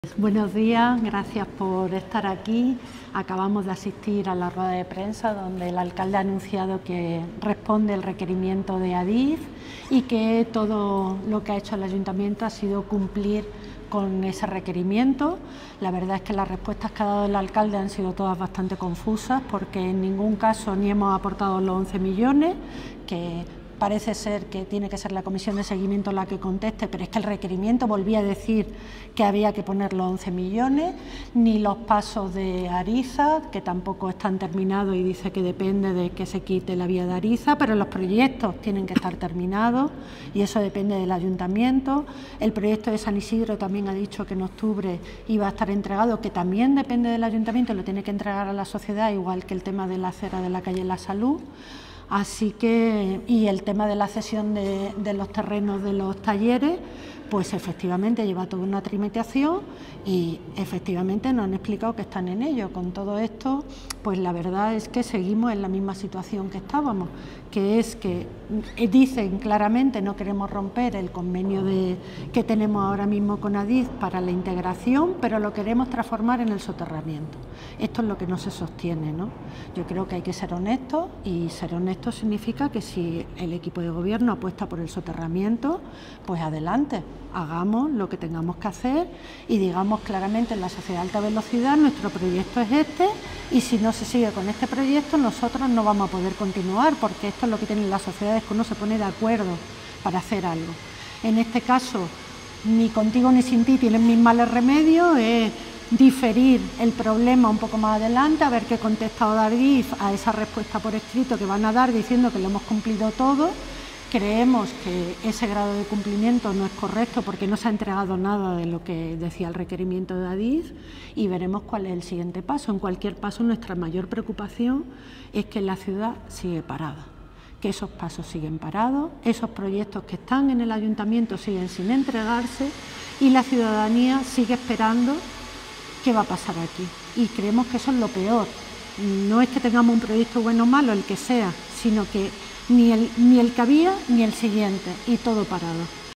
Rocío Anguita critica la política de dilación del ayuntamiento de Valladolid
Rocío Anguita critica la falta de avance en el convenio de integración ferroviaria y la política de dilación del ayuntamiento de Valladolid. Valladolid Toma la Plaabra exige transparencia y acción inmediata ante la paralización de proyectos ferroviarios en Valladolid.